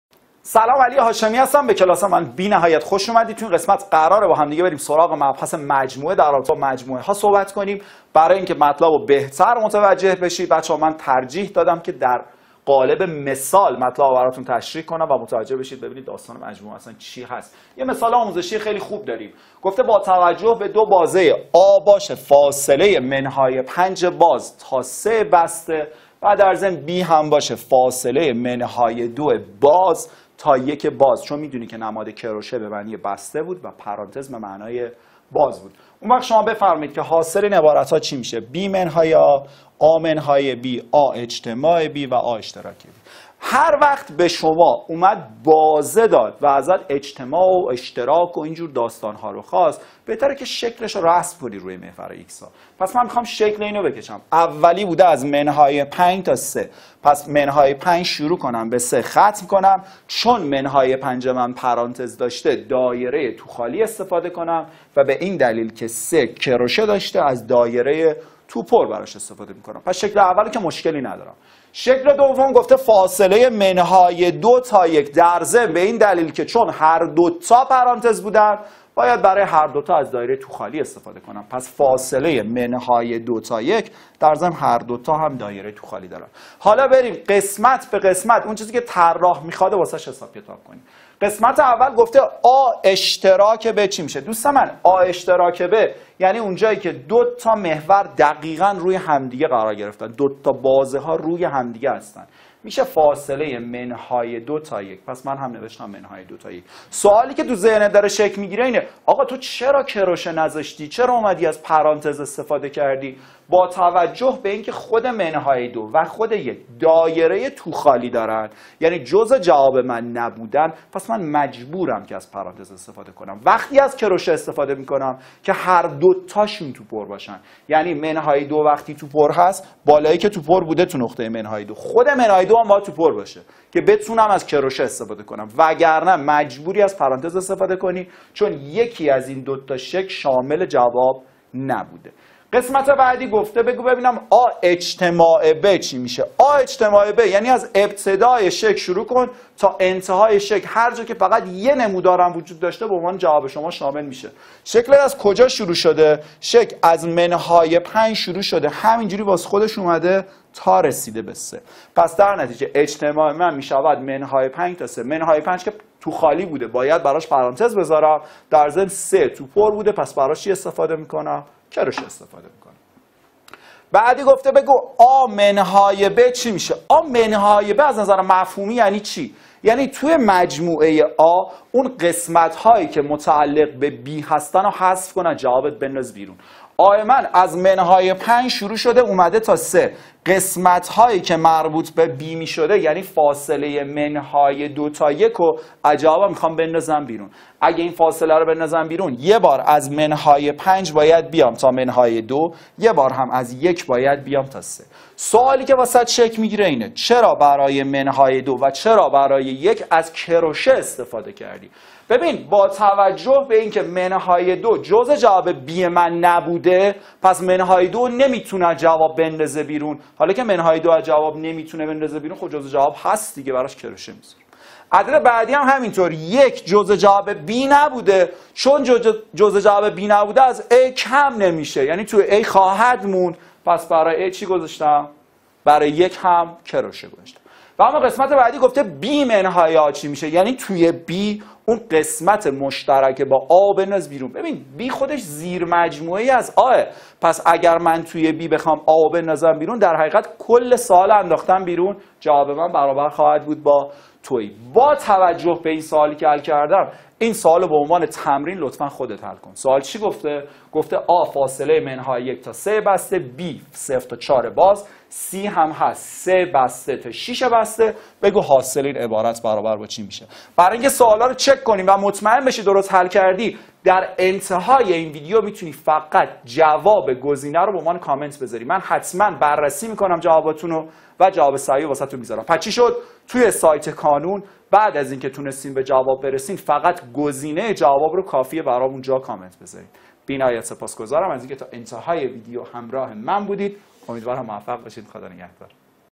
آموزش ریاضی دهم تجربی علی هاشمی فصل اول درس 6 مجموعه ها
در این فیلم، آموزش ریاضی دهم تجربی علی هاشمی فصل اول درس 6 مجموعه ها را خواهید دید.

این ویدیو از پکیج تدریس فصل 1 ریاضی دهم تجربی سایت علی جبرا انتخاب شده است.

برای دانلود فیلم های (درسنامه و جزوه pdf) فصل مجموعه ها، الگو و دنباله کتاب درسی ریاضی دهم تجربی در سایت علی جبرا بر روی لینک زیر کلیک کنید.

اگر سوالی داشتید حتما در قسمت نظرات بیان کنید.